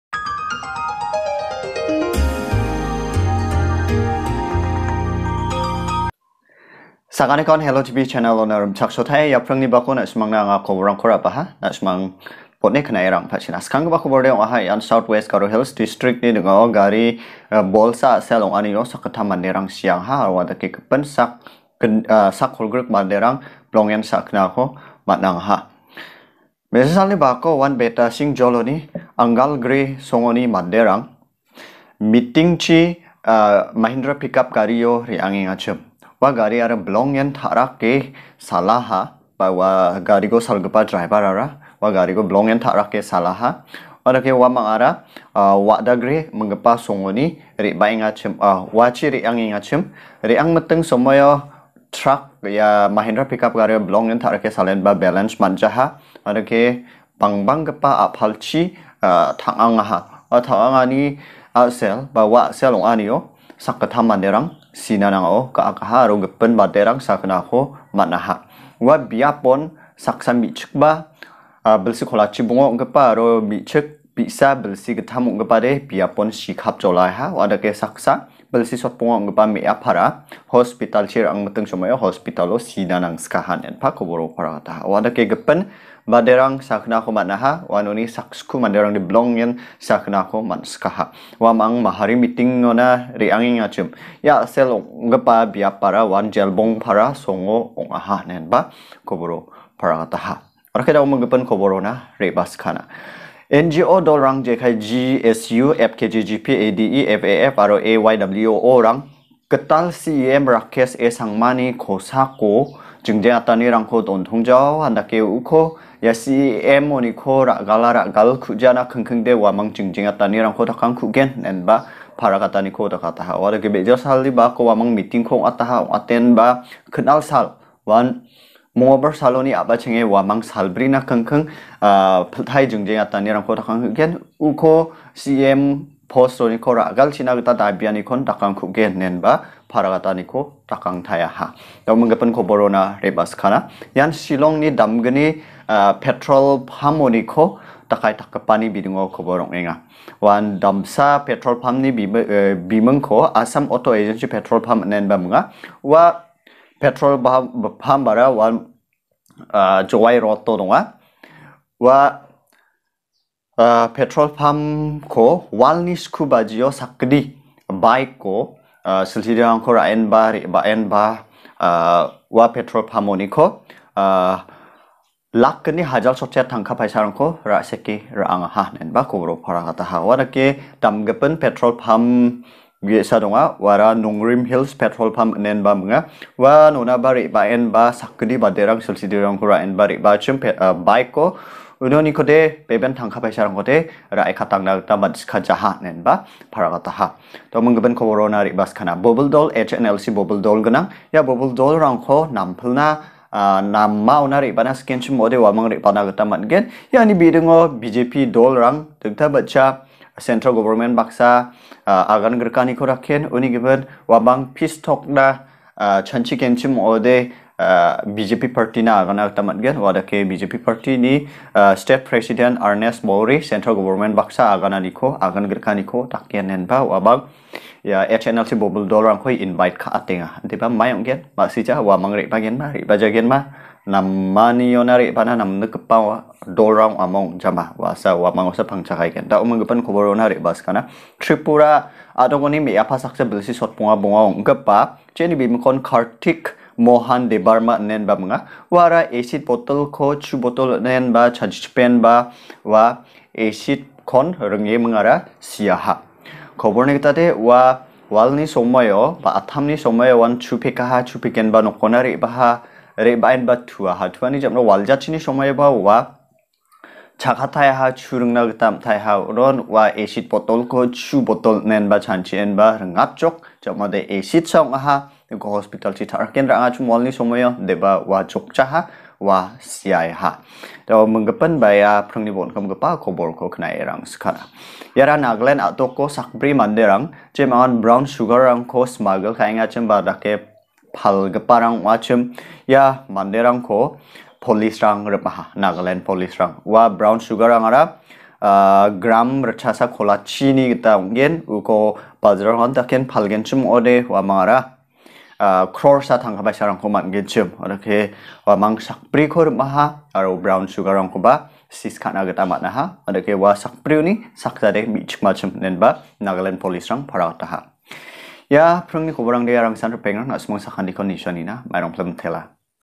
Gari A'sel ong'e Sak.3 sia, CEMko Ra'galchina Dabiangkugen, Acid Ringe Sia. HTV
NB: Representatives photos are used in the Video.